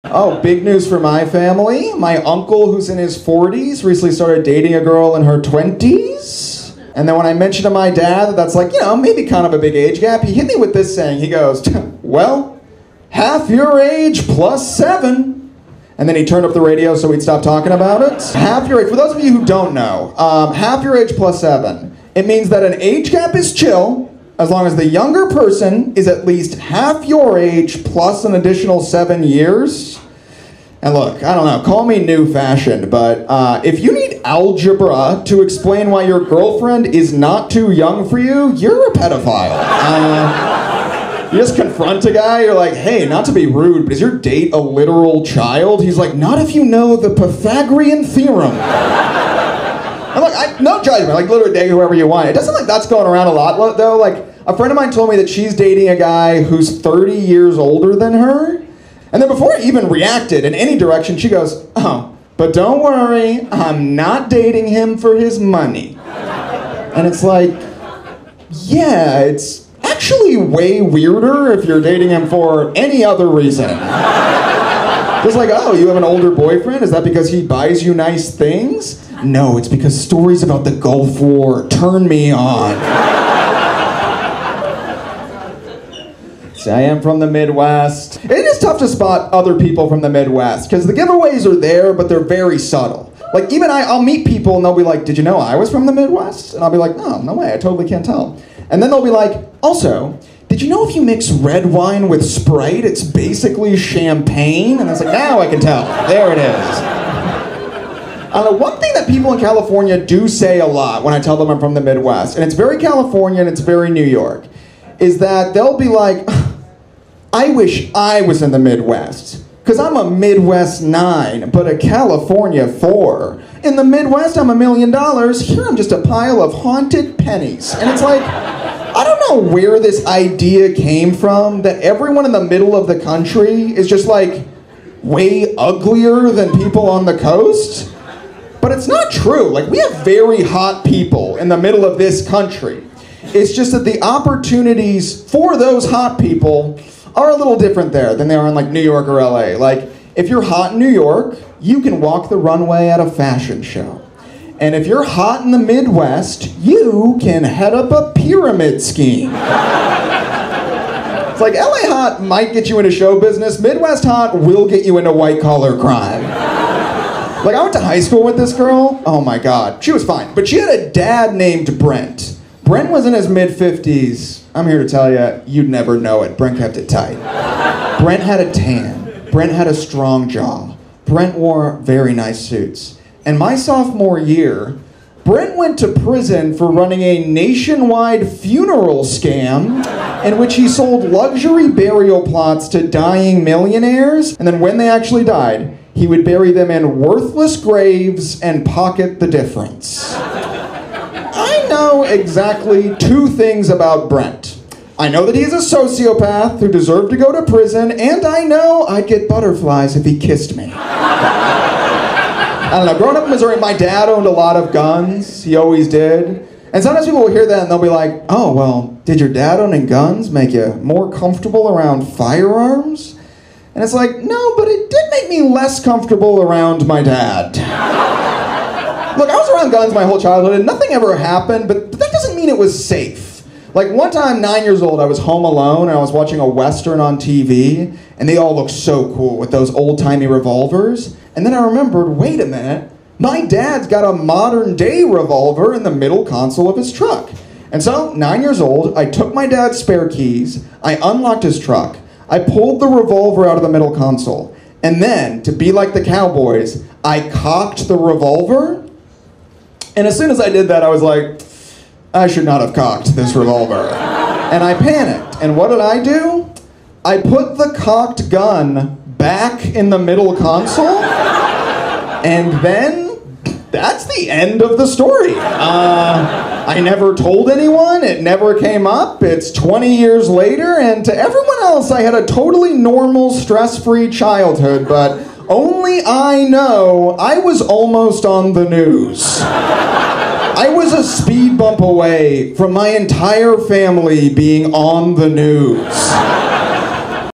Let Hot People Do Crimes - Robert Schultz | Standup Comedy
Robert Schultz talks about the weird "half your age plus 7" rule and about how hot people in the Midwest deserve more respect